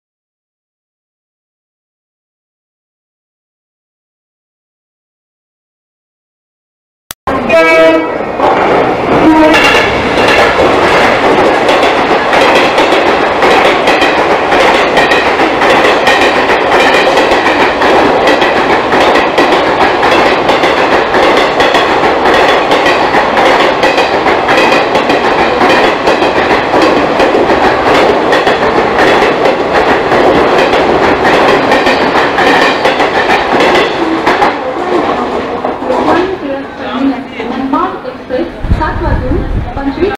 11302 Udyan Exp strikes Thane led by regular KJM WDP4
Valentine Gift from EMD monster KJM WDP-4.

As i got down at Thane and arrived at Platform 7, i heard  a loud honk which was of none other than WDP-4 of KJM shed gushes through Platform no 6 of Thane with 16530 SBC-CSTM Udyan exp
Speed was somewhere around 90-95 kph